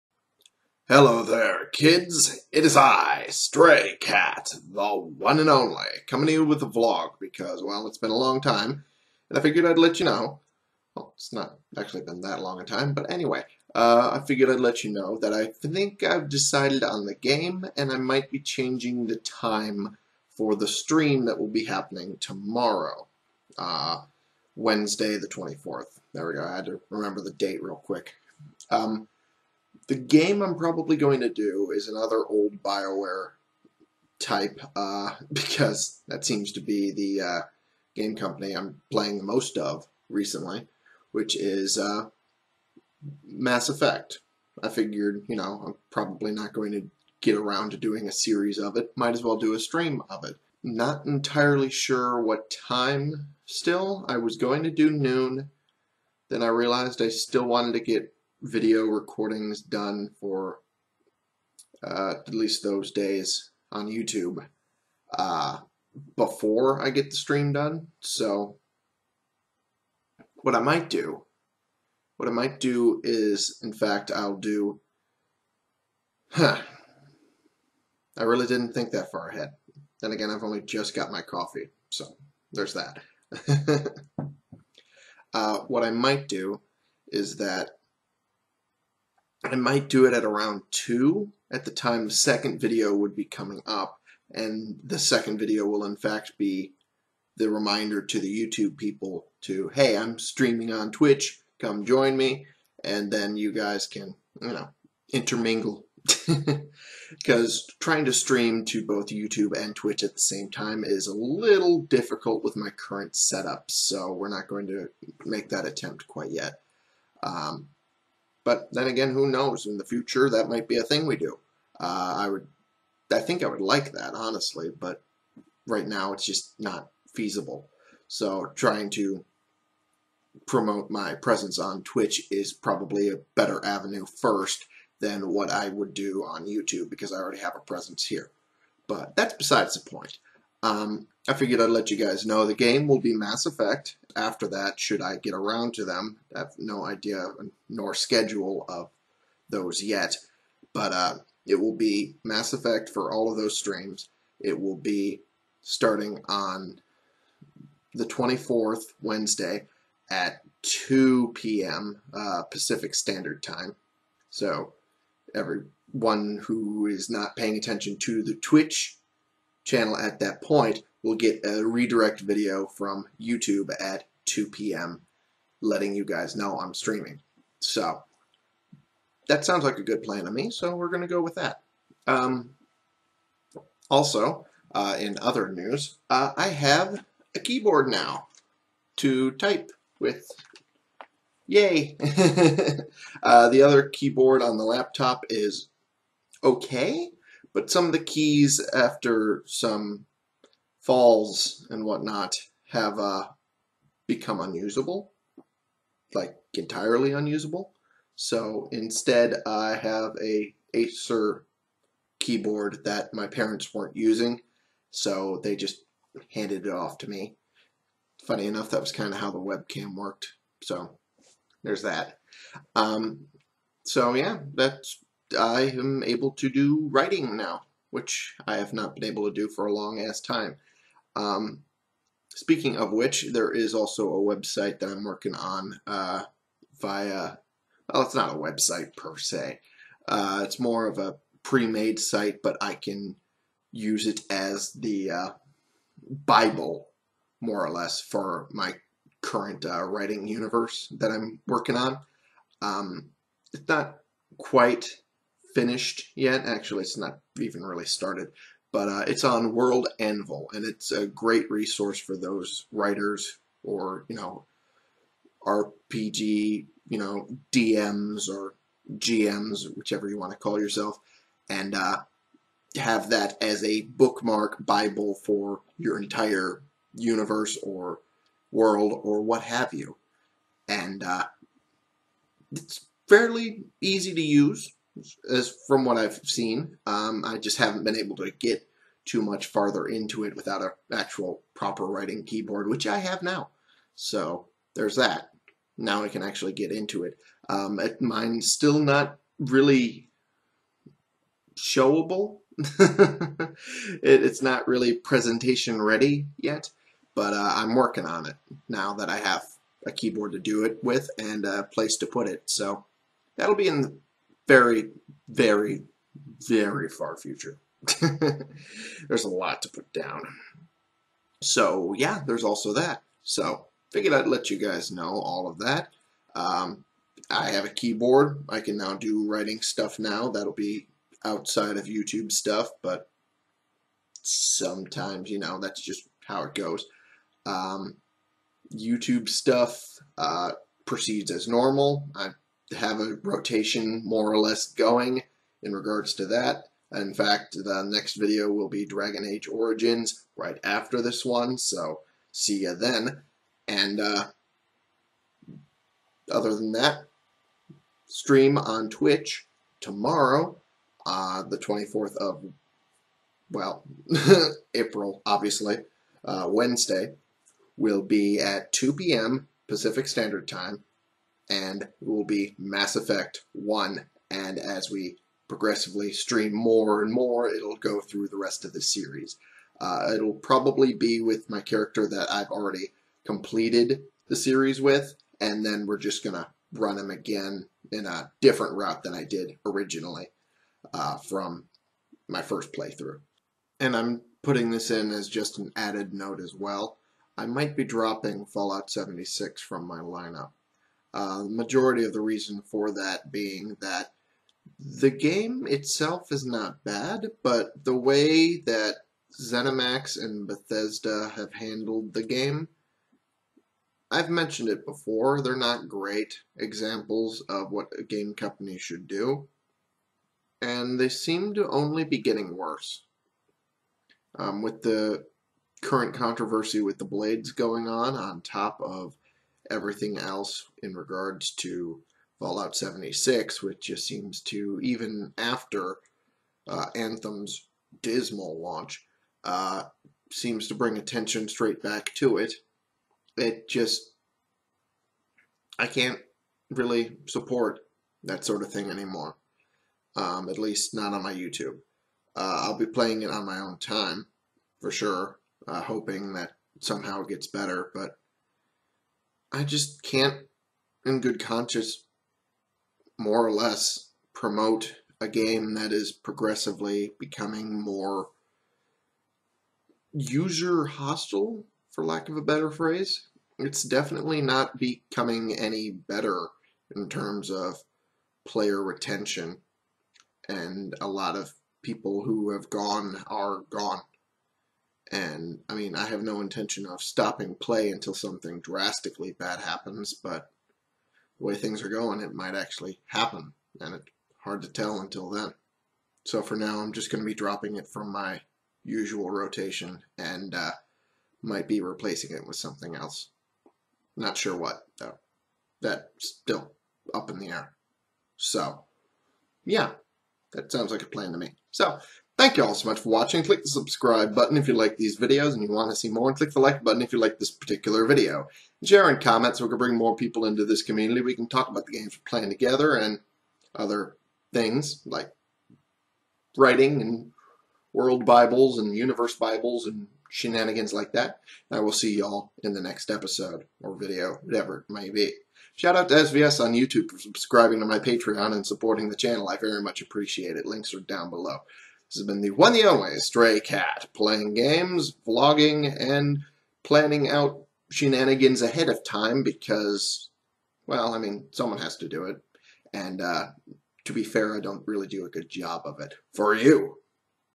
STREAM GAME AND TIME ANNOUNCEMENT TOMORROW And Writing Tool Get!
I mention the plan for tomorrow's stream, the game changes on the channel in general and what the stream time and game will be! I also talk about my new typing keyboard (which I'm using now) as it pertains to outside of YouTube work!

Shoutout to SVS on YouTube for supporting the channel!